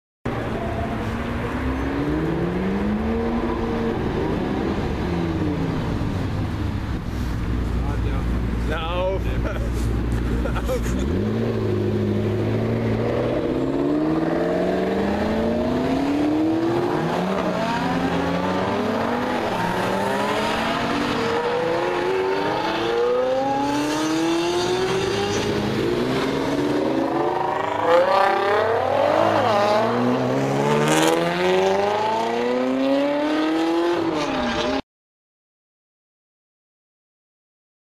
R8 V10, 360 CS, 12C spider and GT-R lovely sounds HD
in this video you can see a nice Audi R8 V10, Ferrari 360 Challenge Stradale, McLaren MP4-12C Spider, Nissan GT-R and some other cars. I hope you like this video.